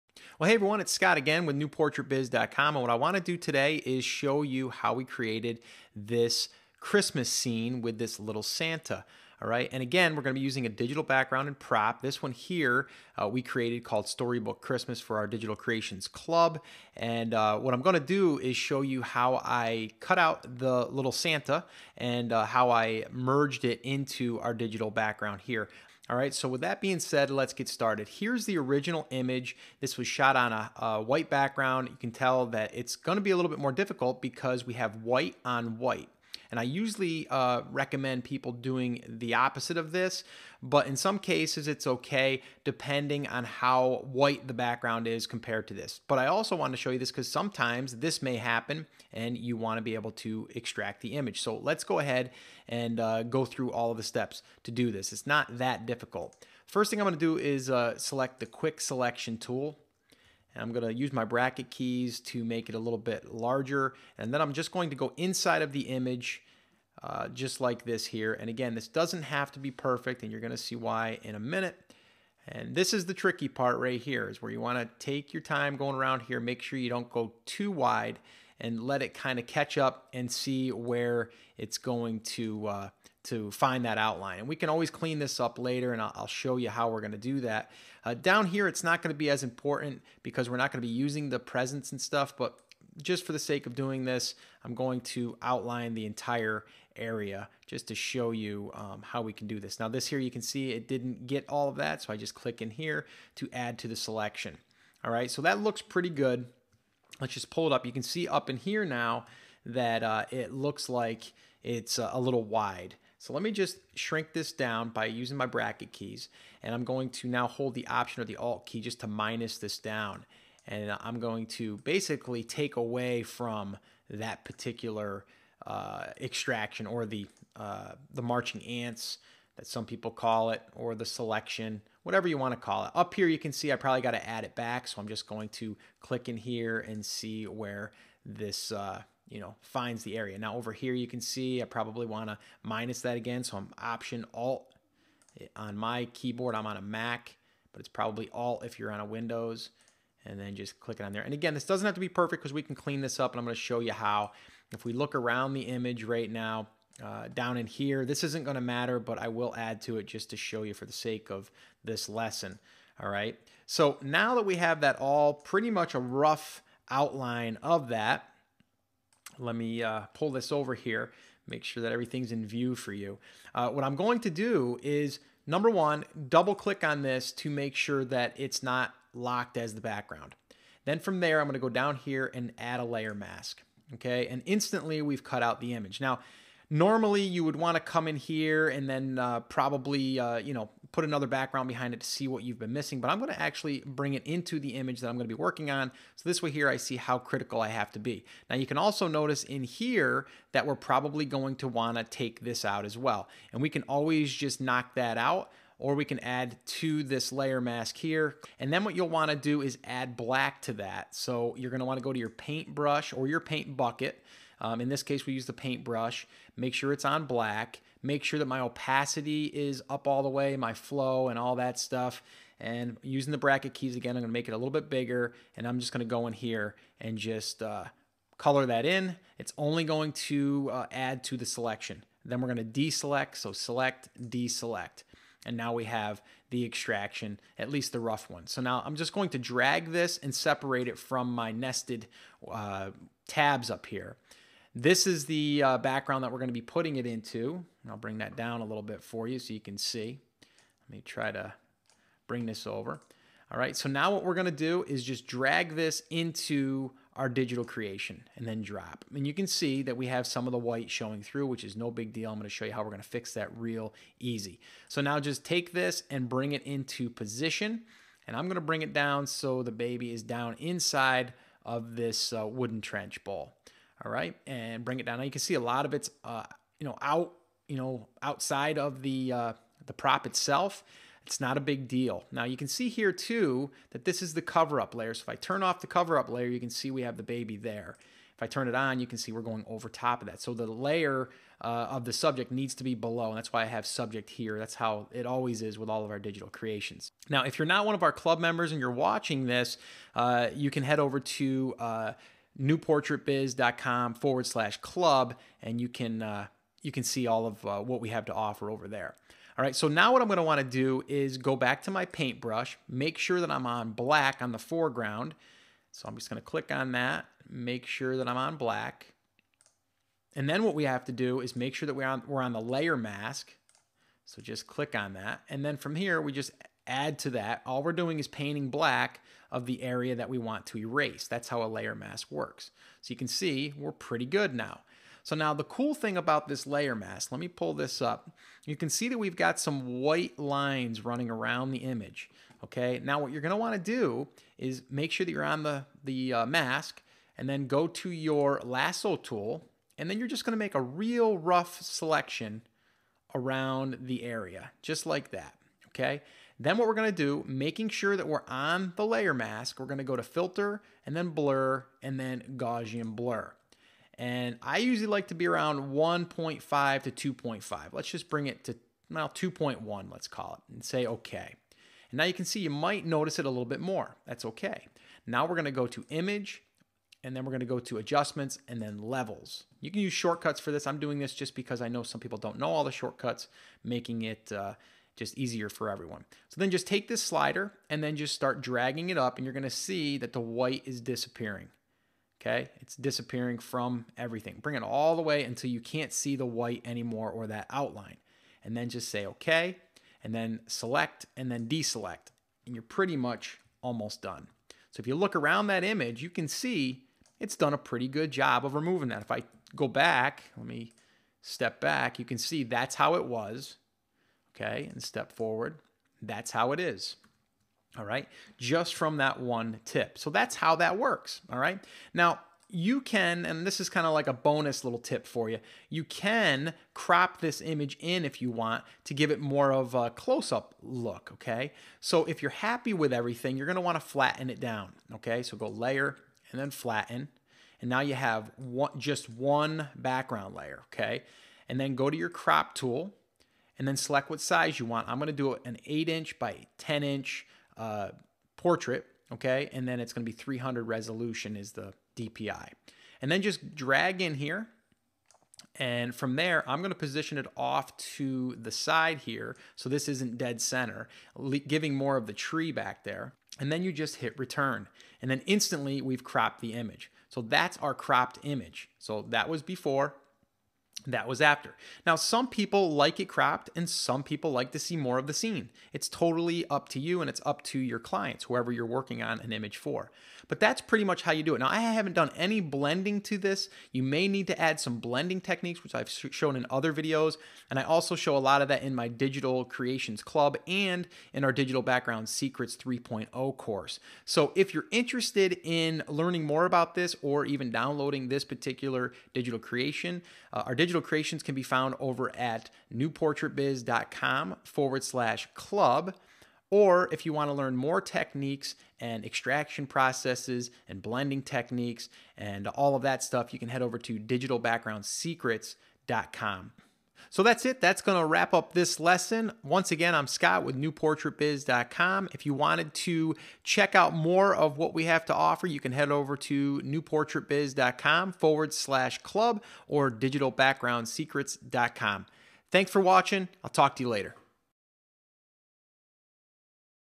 Using Digital Backgrounds and Photoshop To Create Christmas Portraits
- In this lesson you will learn how to create Digital Backgrounds and Props with the help of Photoshop. Using the Quick Select tool and layer masks to extract your subjects.